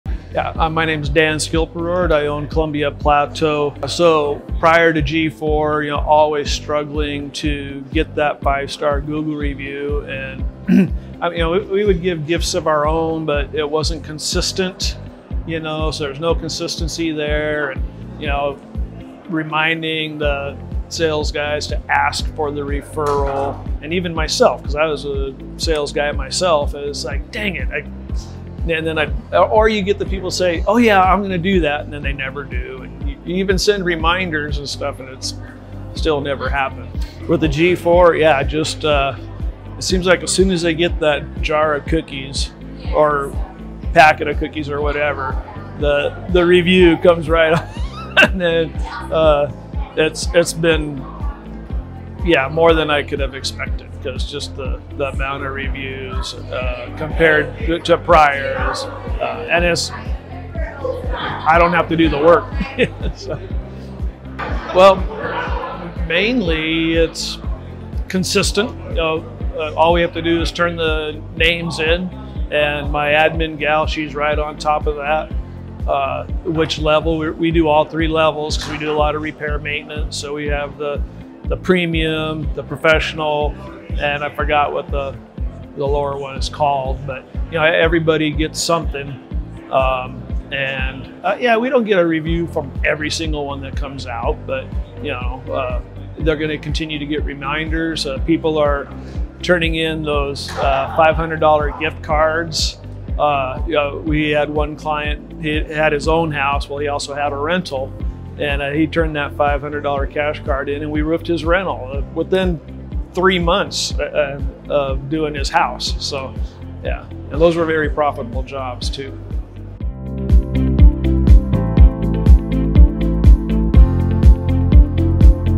Dan Schilperoort gFour Testimonial
Unleash the Power of Your Own Customers

Hear what Dan has to say about gFour's done-for-you relationship marketing system that transforms the customers of busy home improvement and home services companies into Raving Fans that will generate 5-star reviews, ongoing referrals, and repeat business.